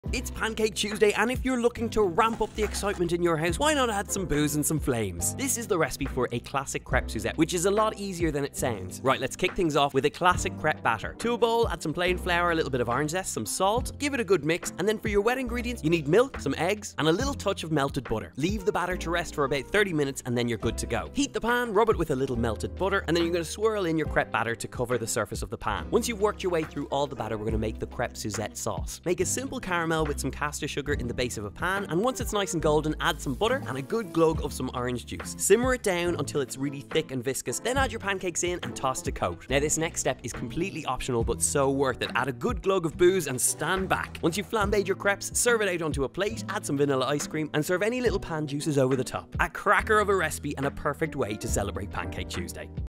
Crepes Suzette 🥞🍊
Crepes Suzette | Pancake Tuesday is next week and if you want to up your pancake game, Crêpes Suzette is one of those classic showstopper desserts to try 🔥🥞 Remember to be cautious with the flambé part by having your head back and keep a lid handy just in case the fire gets too big!

For the pancake batter:
100g plain flour
Zest 1 large orange
Pinch of salt
225ml of milk
2 free-range eggs
25g butter, melted, plus extra for frying

For the sauce:
50g caster sugar
50g butter
Juice of 2 large oranges
4 tbsp cointreau orange liqueur triple sec

Sieve the flour into a large mixing bowl, add the orange zest, pinch of salt and with a spoon, make a well in the centre. In a separate bowl add the milk, eggs and melted butter and mix until combined. Pour the milk mixture into the flour well and slowly incorporate. Whisk until you have a light and smooth batter.
Add a little butter to your frying pan over a hot heat, evenly coat the surface and then, moving quickly, add a ladleful of the batter to the hot pan and move from side to side until it evenly covers the surface of the pan. Reduce the heat and cook for about a minute each side or until the batter begins to take a nice golden colour. Fold in half and then in half again to create a triangle and keep warm. Repeat this with the remaining batter (you should get 4 pancakes total).
To make the sauce, add the sugar into the pan and let it caramelise a little but not turn into a caramel colour. Add the butter, swirl through and once frothy, add the orange juice to the pan. Once the sauce is slightly thick, add the pancakes back to the pan, one at a time, coating in the juices as you go. Continue like this until they're all re-heated, folded and well soaked with the sauce.
Drizzle the grand marnier into the pan, tilt the pan towards the flame to flambé or use a lighter, taking care at this point of the flames. Serve onto individual plates with a drizzle of any remaining sauce in the pan and a scoop of ice cream.